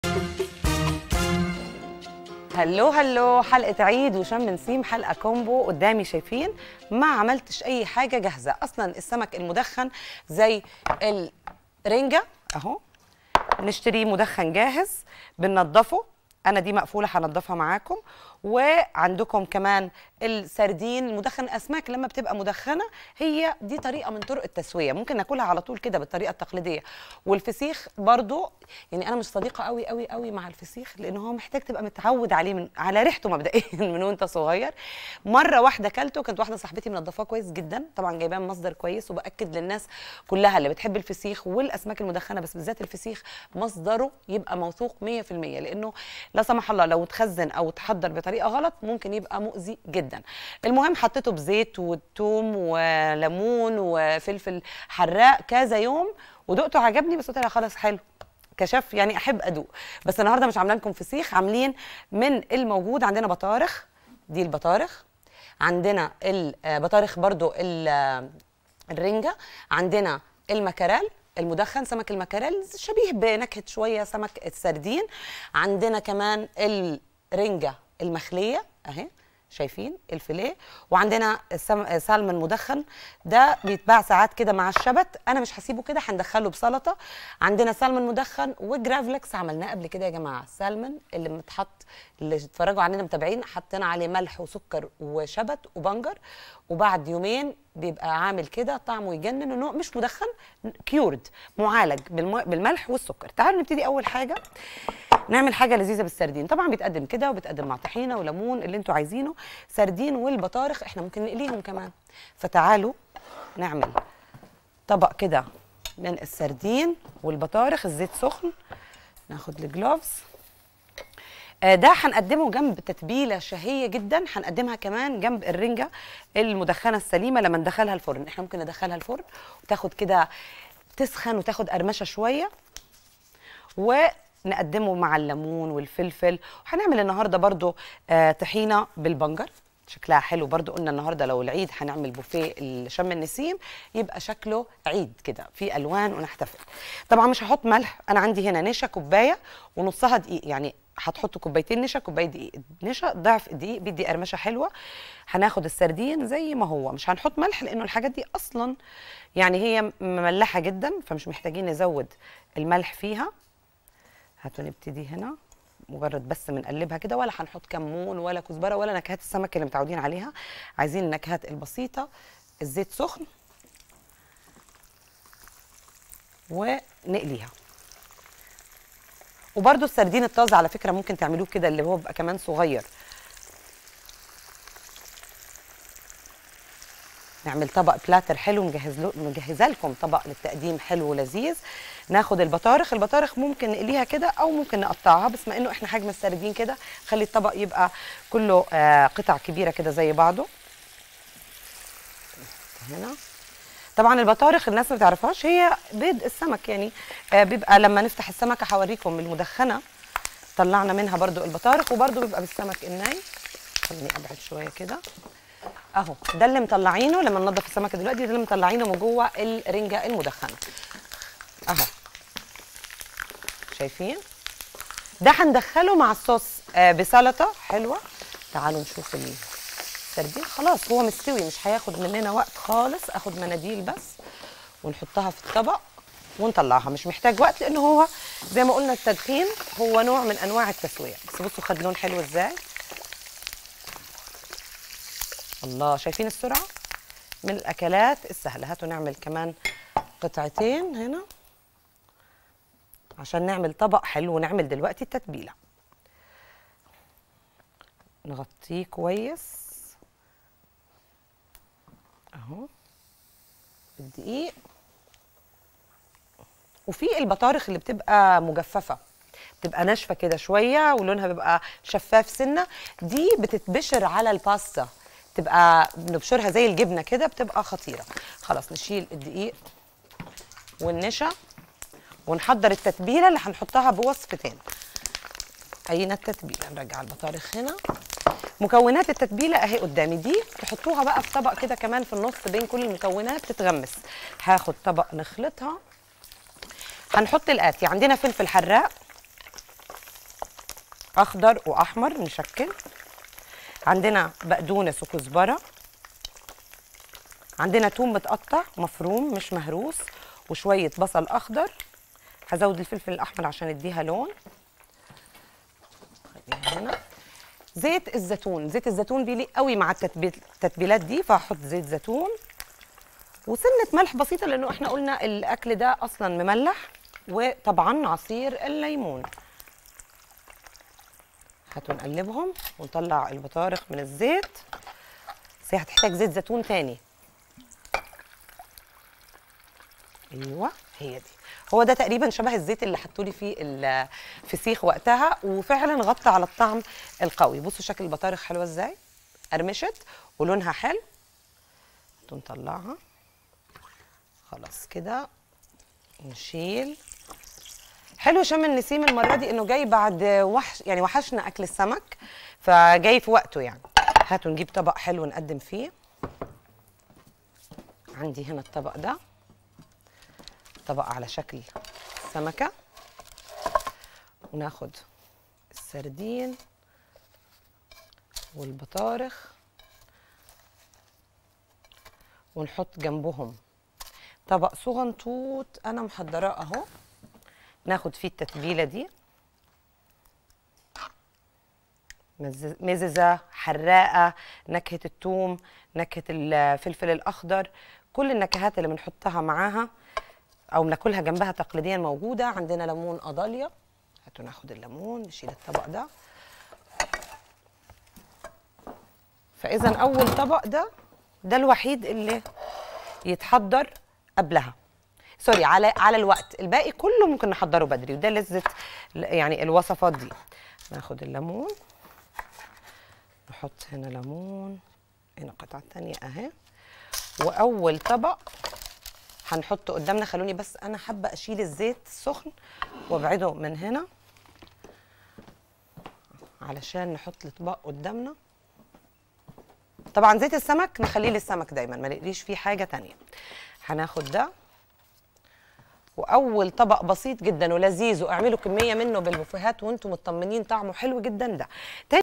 مي شو مع مي يعقوبي| طريقة عمل سردين وبطارخ مع الصوص
١- السردين والبطارخ
٢ كوب نشا
كوب دقيق
ملح
سردين
بطارخ